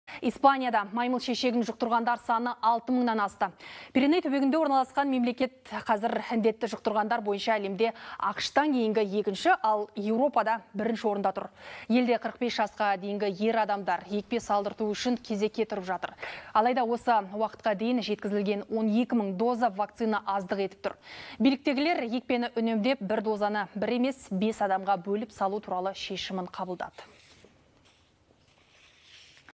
Испанияда маймыл шешегін жұқтырғандар саны 6 мыңнан асты
Пиреней түбегінде орналасқан мемлекет қазір індетті жұқтырғандар бойынша әлемде АҚШ-тан кейінгі екінші, ал Еуропада бірінші орында тұр. Елде 45 жасқа дейінгі ер адамдар екпе салдырту үшін кезекке тұрып жатыр. Алайда осы уақытқа дейін жеткізілген 12 мың доза вакцина аздық етіп тұр. Биліктегілер екпені үнемдеп, бір дозаны бір емес, бес адамға бөліп салу туралы шешім қабылдады.